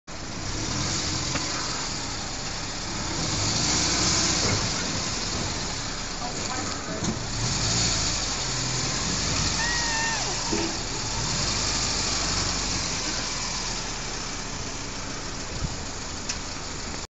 Baby Co-pilot
Sammi is out the window guiding the car backwards :)